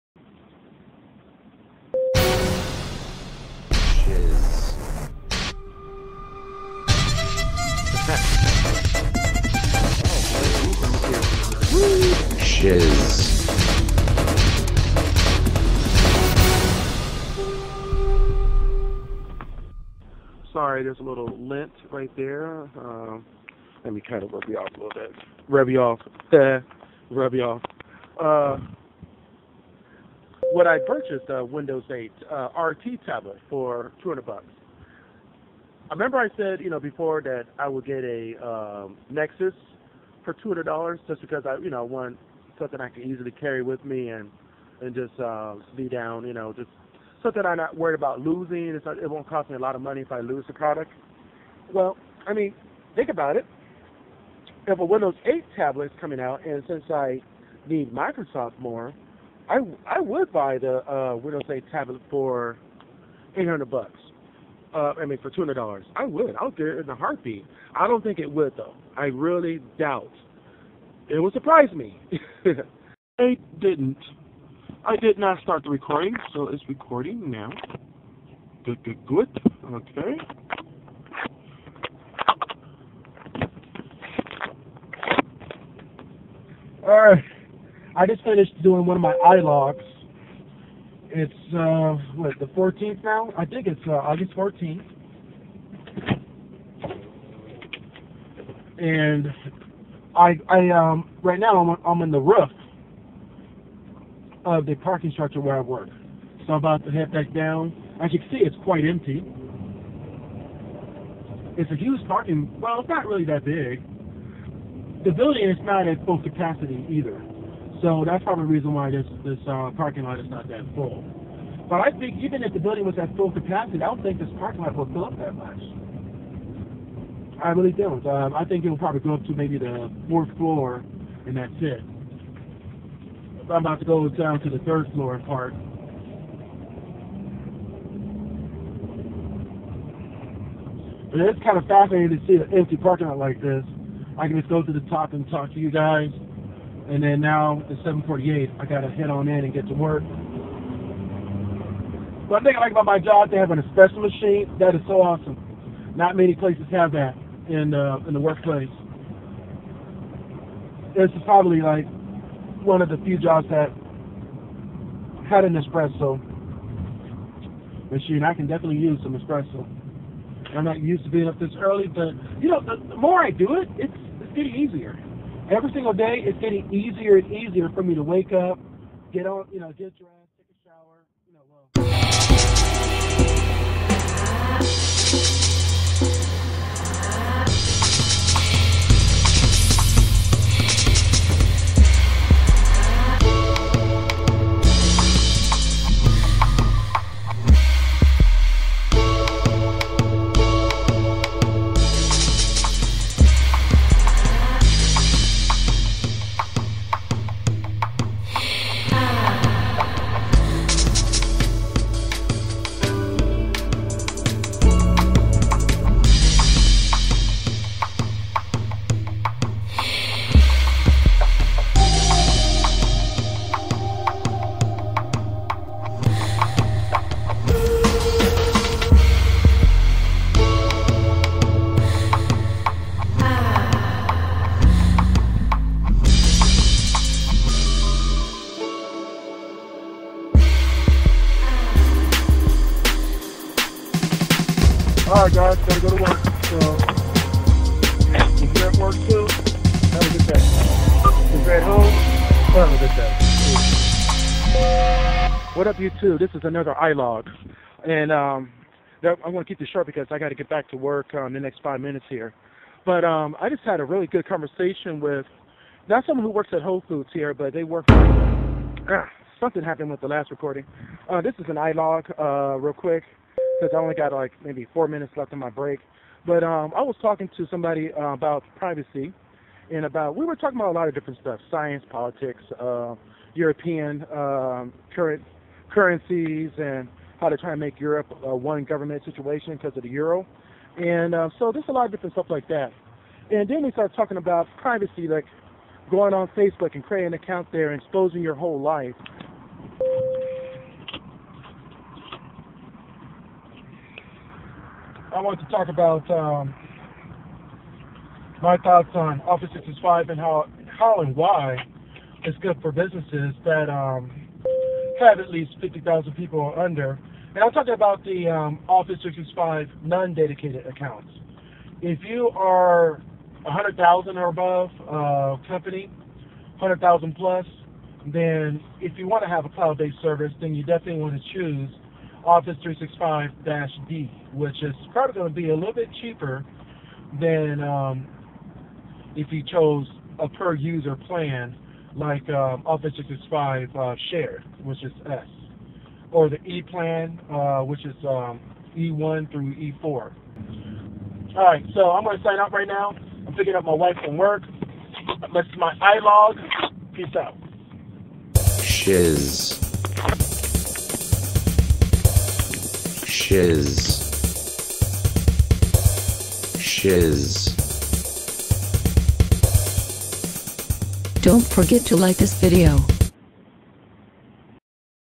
vlog 20120815: mini-shiz - iLOG Microsoft Privacy Roof Carport (show ms-imprc)
Show ms-imprc: On this day, I did a lot of ilogging, which is the new short logs. This vlog showcases excerpts from the ilogs.

iLOGs

Here are links to the ilogs that are covered in this vlog.

1. sHiZ iLOG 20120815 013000: Webcam video from August 15, 2012 1:03 AM

2. sHiZ iLOG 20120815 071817: Windows RT for $200

3. sHiZ iLOG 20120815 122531: Privacy and Freedom is not Free

4. sHiZ iLOG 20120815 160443: Office 365

Office 365

sHiZ Moments

If you want the music featured in the show, go

Conclusion

CREDITS:
Video produced by and music arrangement by Antonio "sHiZ" Edward

Filename: shiz-20120815.wmv